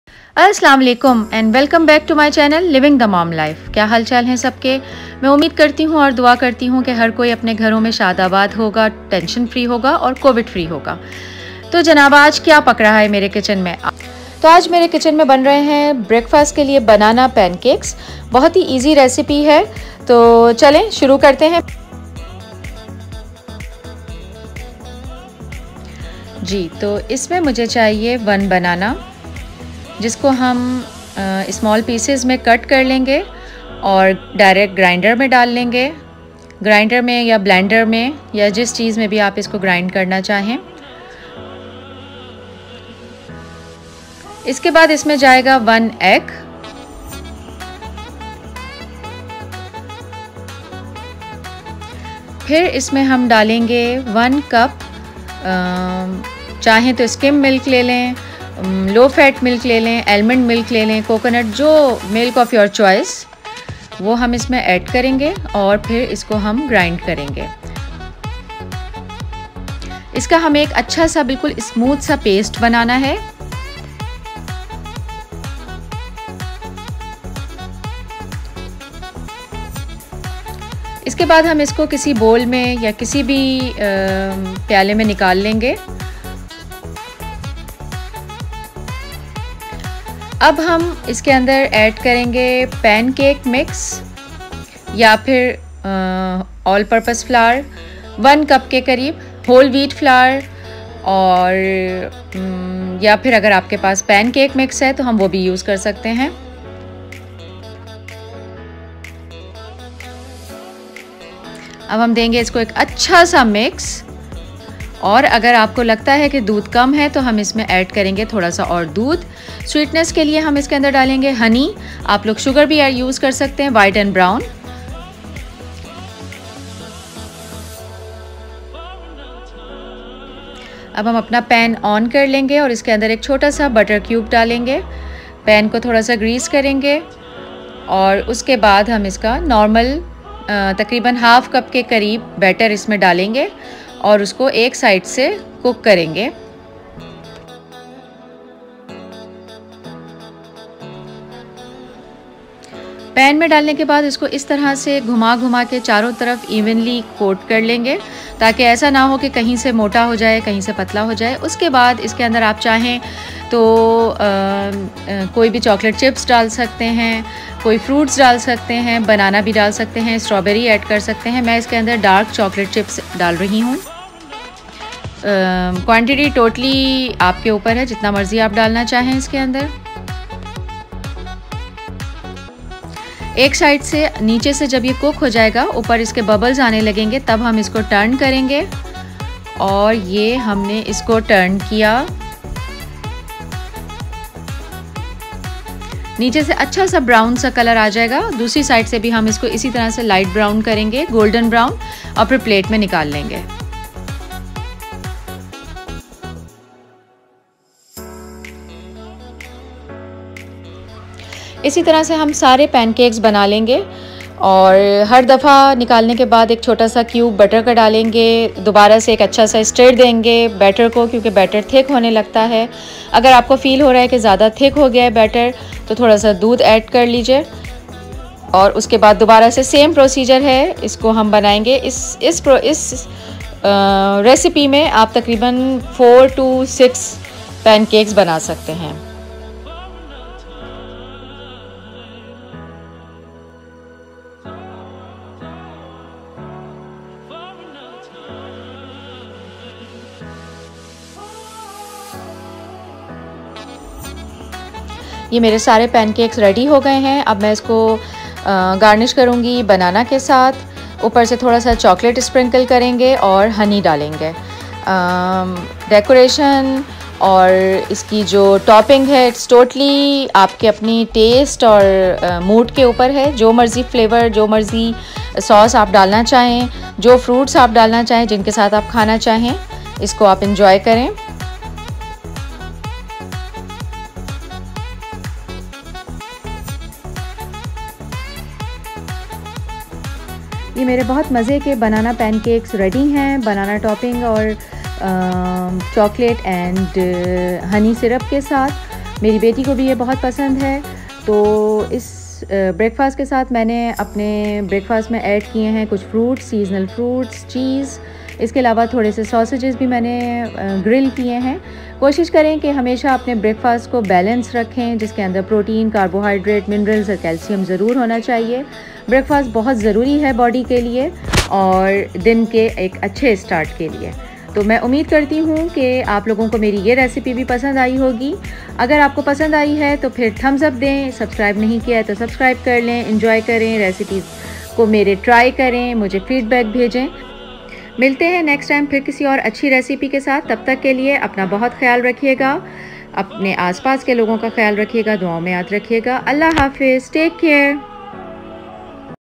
BANANA PANCAKES ARE SO EASY TO MAKE? | RECIPE VIDEO | ENGLISH SUBTITLES
In this video, I'll be showing you a very quick and easy way to make pancakes using bananas. This recipe is super easy to make and the pancakes turn out delicious.